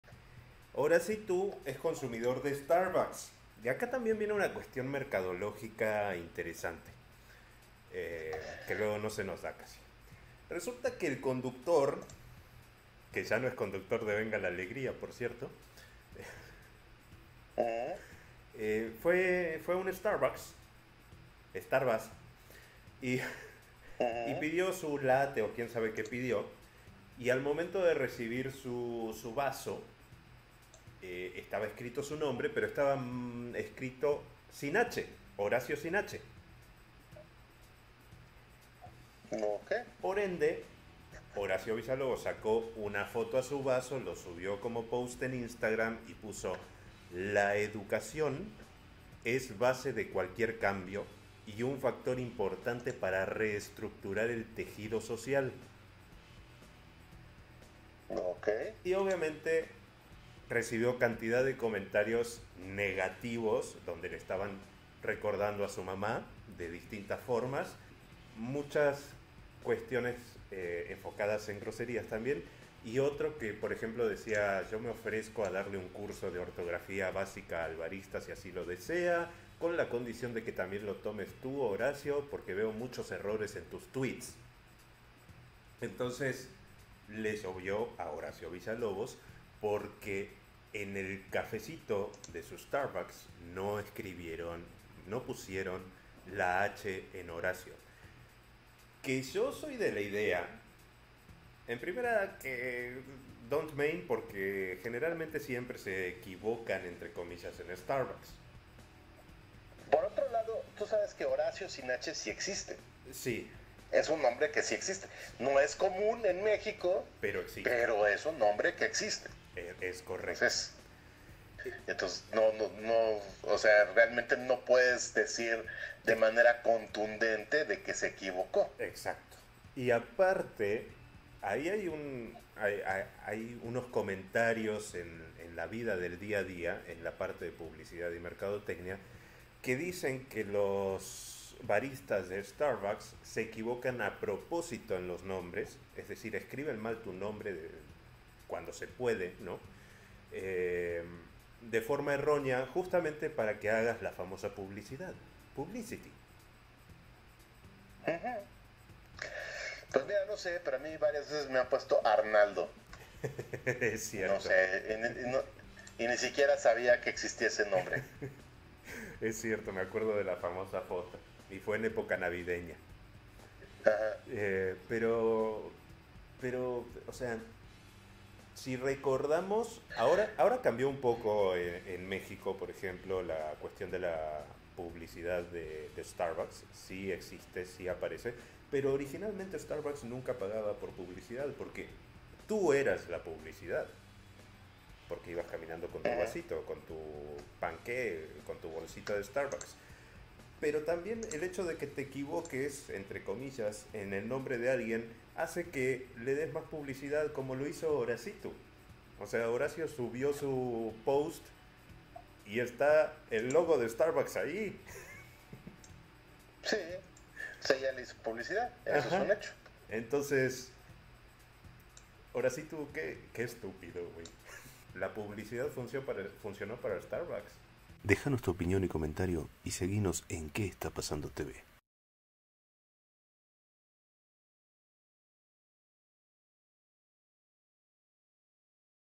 HORACIO VILLALOBOS ARREMETE CONTRA STARBUCKS
Denle un ascenso... pero al barista. Te explicamos lo que ocurrió, quien se enojó y quién perdió... Errar es de humanos...